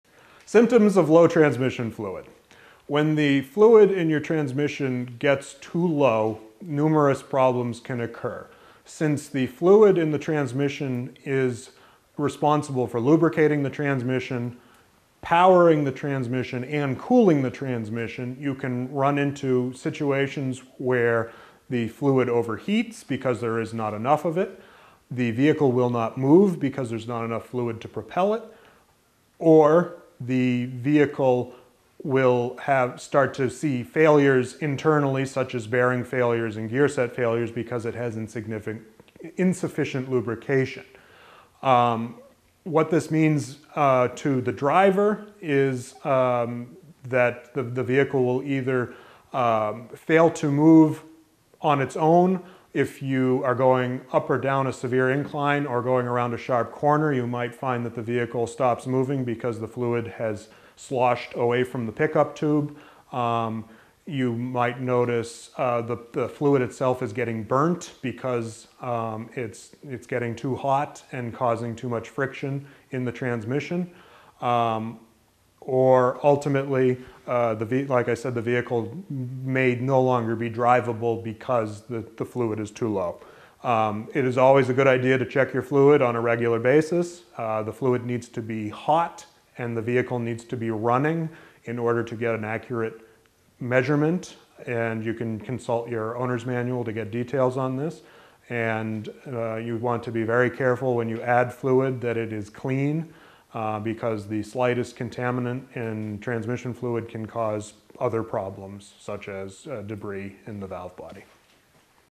Car Maintenance : Symptoms of Low Transmission Fluid
A car with low transmission fluid will stop moving, and other internal failures, such as bearing failures, may occur, as well. Learn about adding transmission fluid to a vehicle correctly with help from an auto mechanic in this free video on car maintenance.

Expert: J.B. Hebert
Bio: J.B. Hebert is a machinist, metal fabricator, and auto mechanic who has been fixing and modifying all things mechanical for over 20 years.